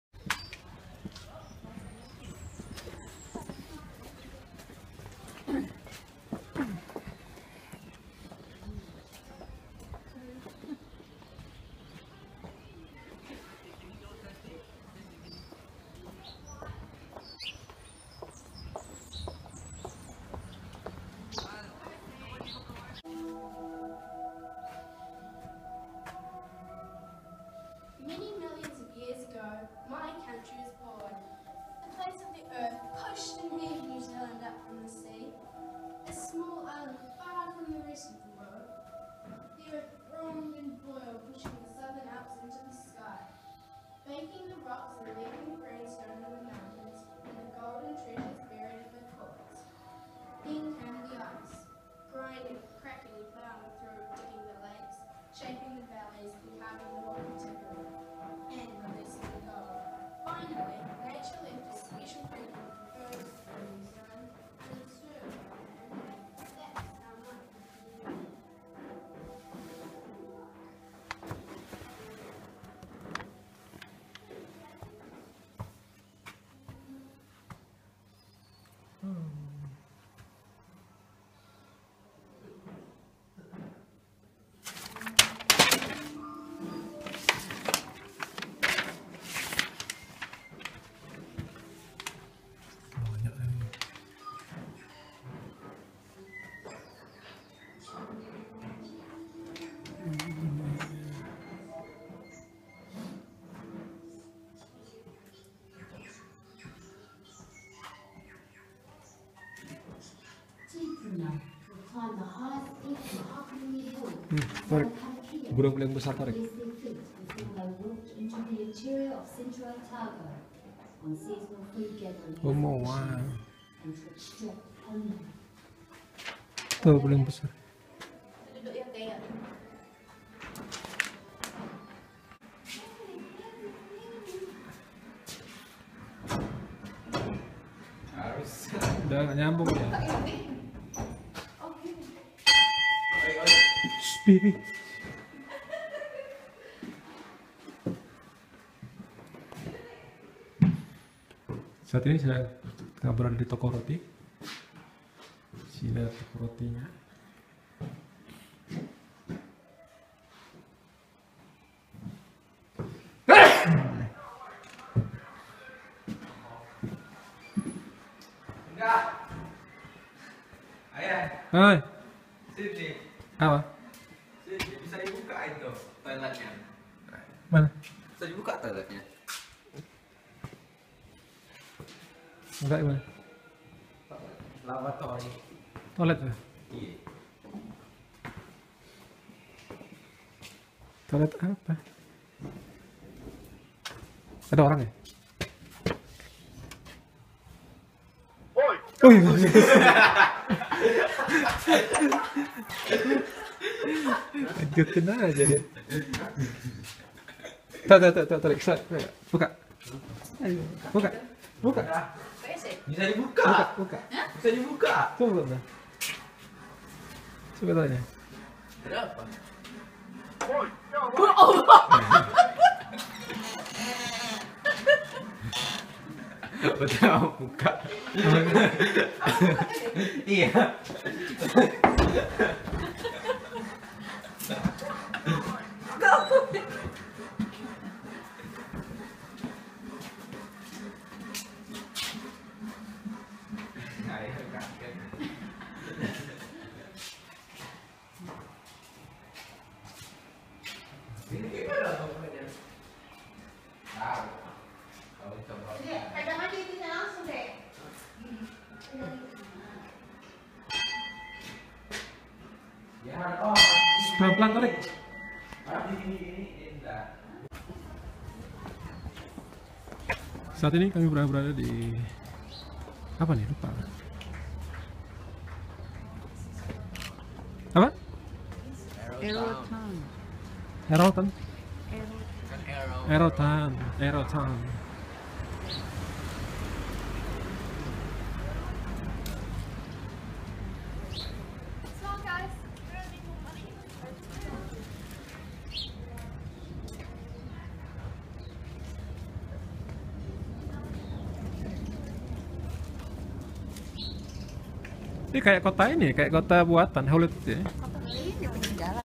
ROAD TRIP NEW ZEALAND 2015 : ARROW TOWN
Arrowtown is a living historic settlement with many stories to tell. Wander by the restored cottages and explore gold mining sights along the Arrow river.